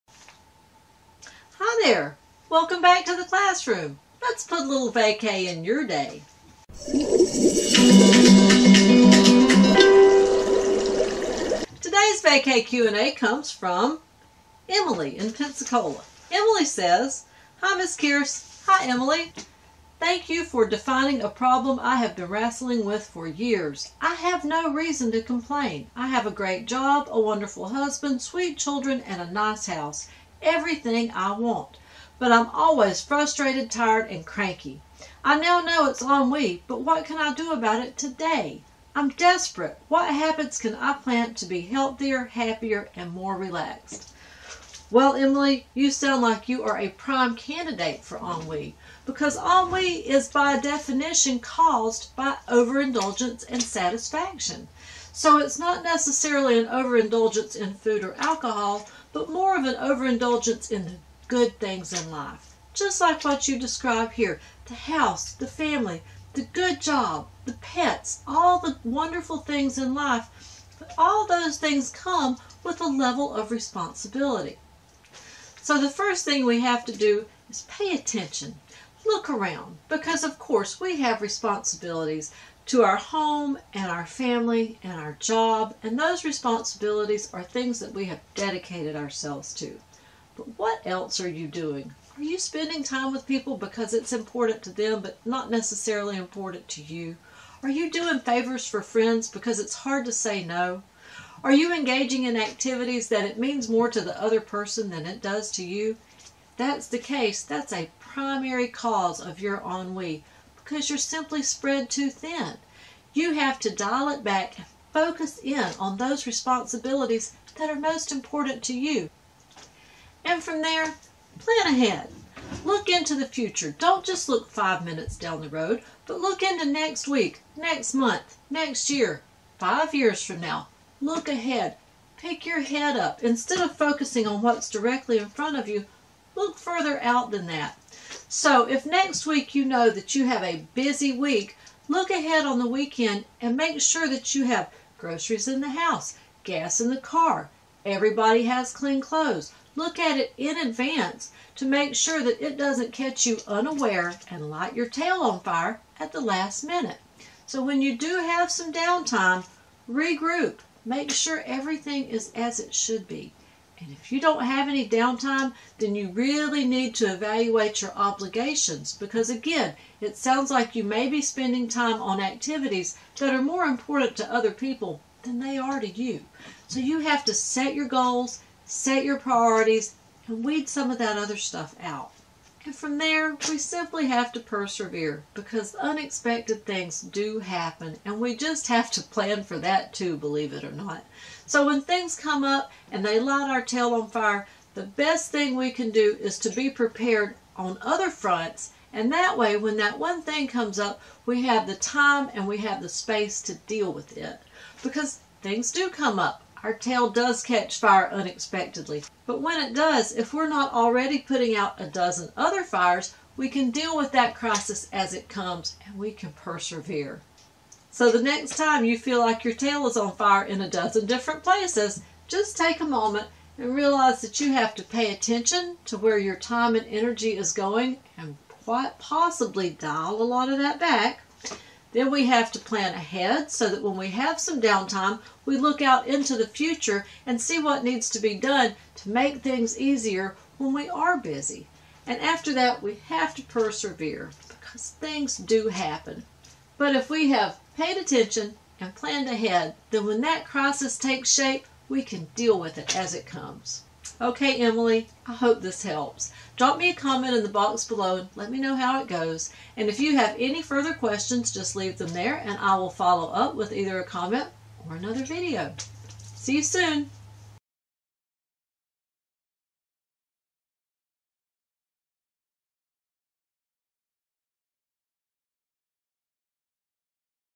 Classroom Q&A: How to be More Relaxed
Question: How can I be healthier, happier, and more relaxed?